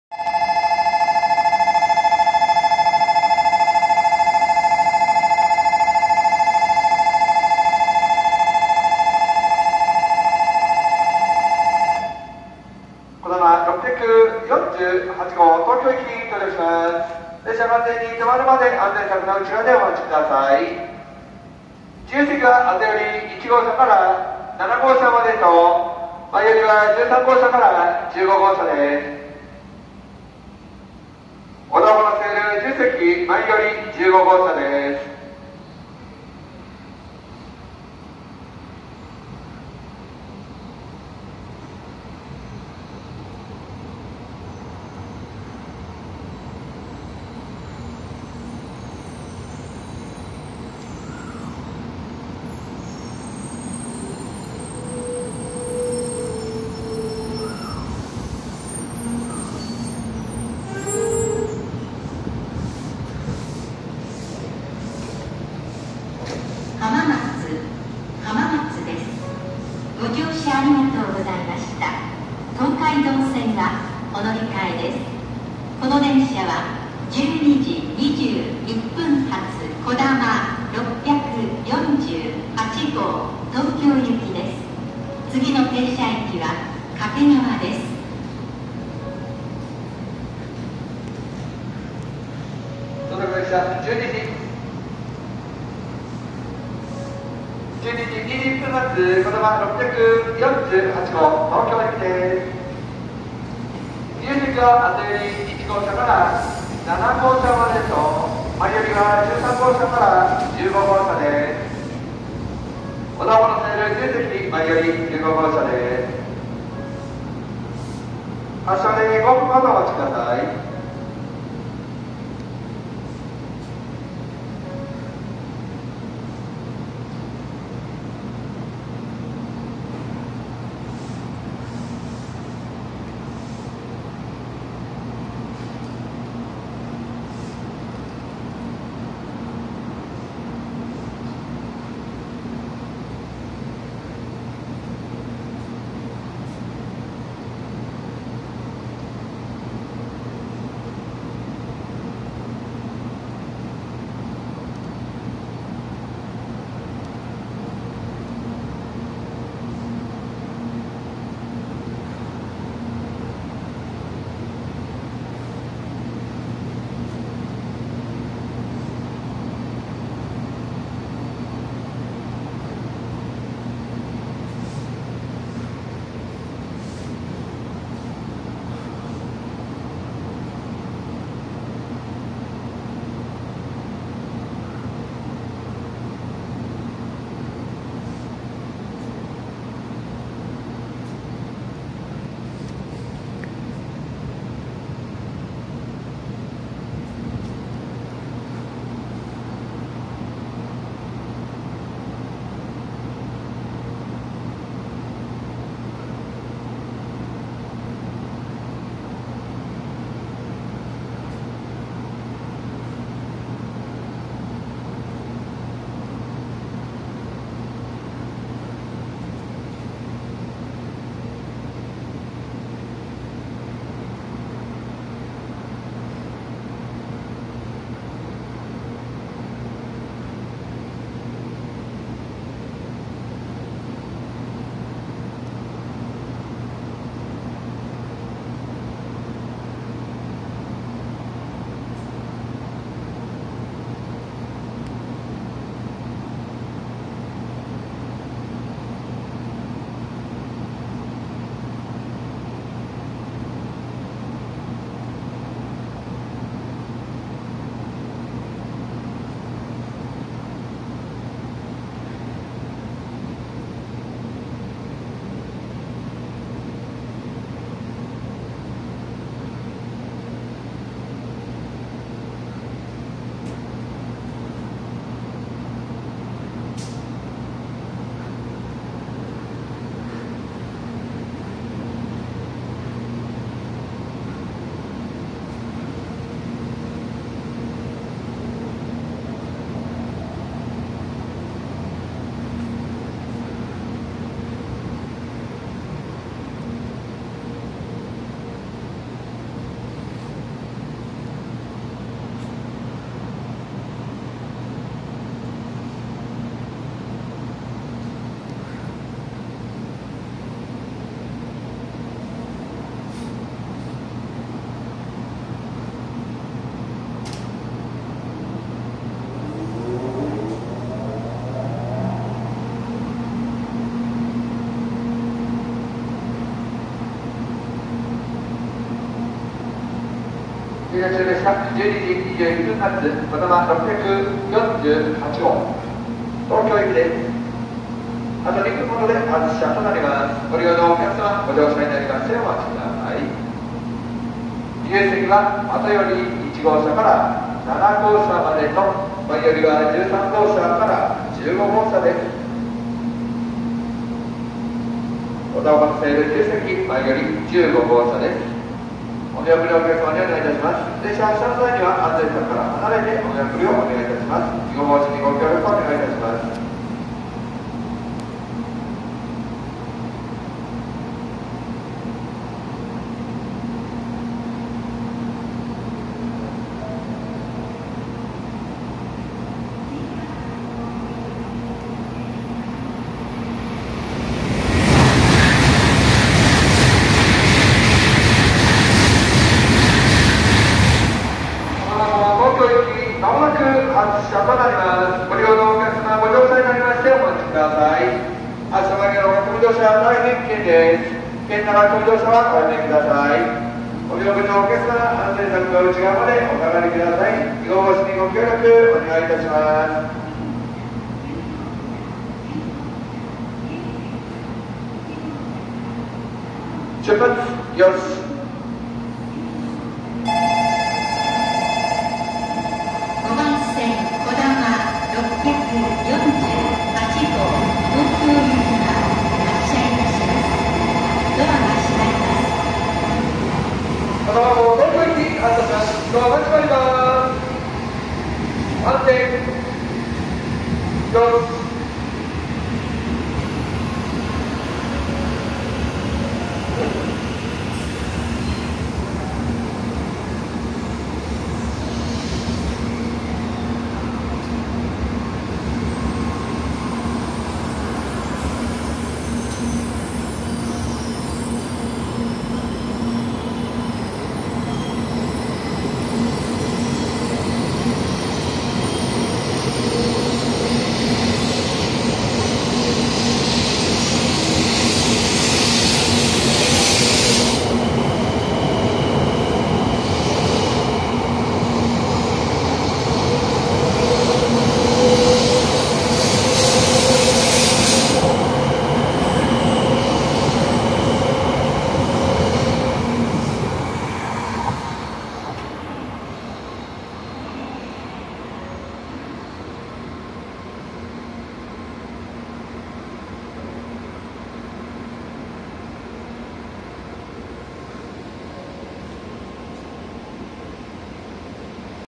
700系新幹線(上り)浜松駅到着～出発まで
700系新幹線を撮影してみました